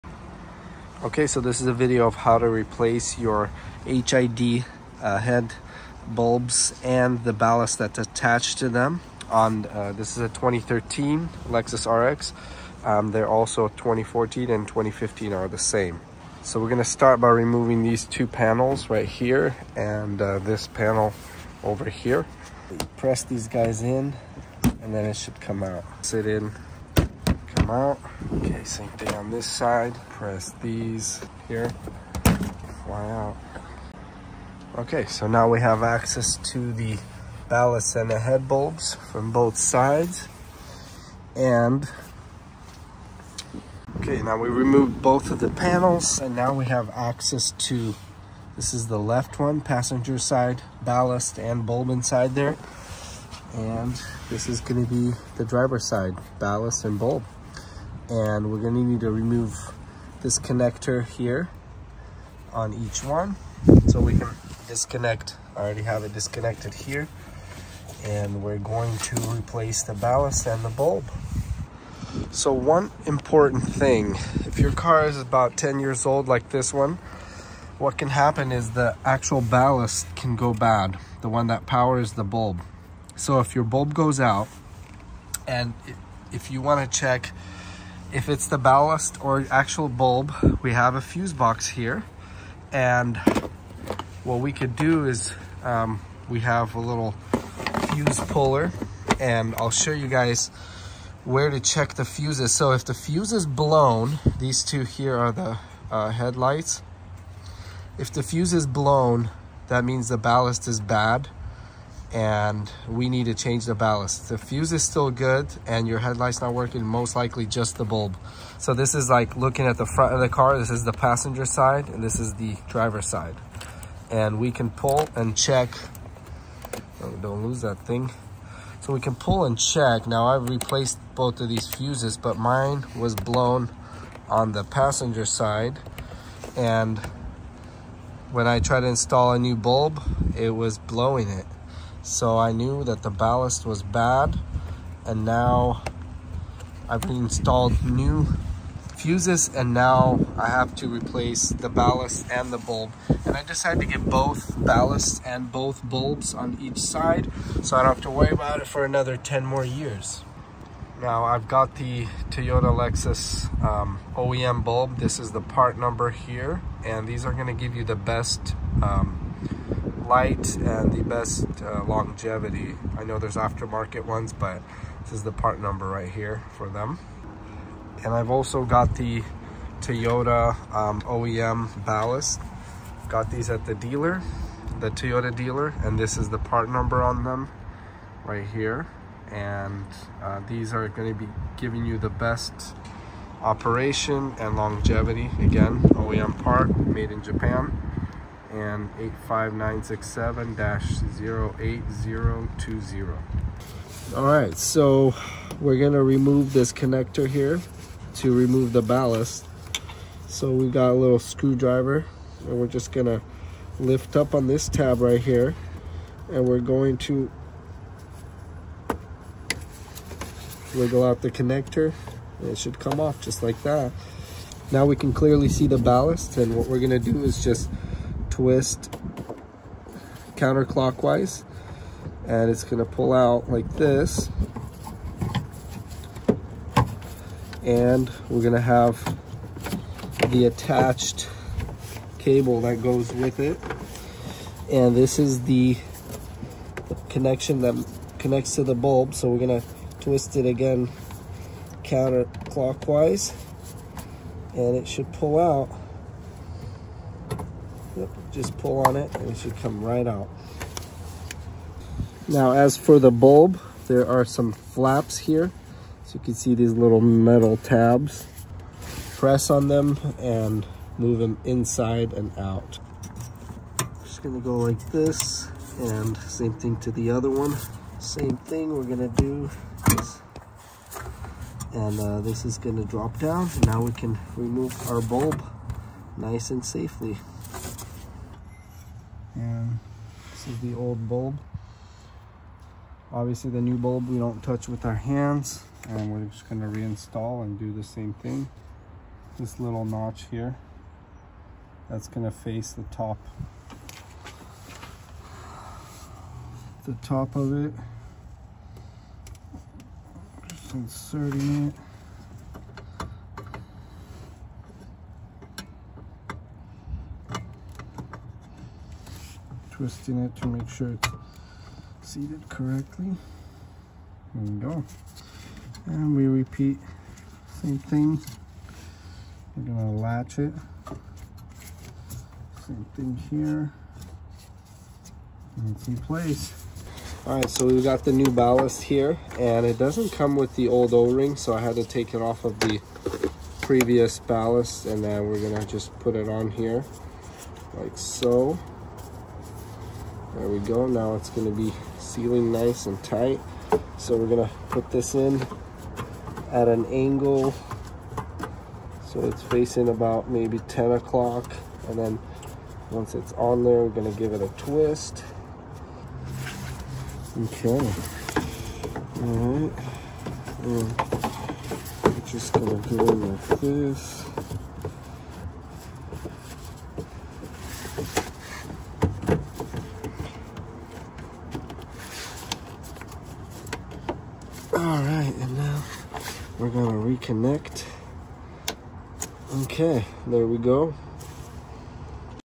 Lexus RX350 HID Headlight Replacement Guide 2013 2014 2015
How to replace a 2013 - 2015 Lexus RX 350 HID Headlight and Ballast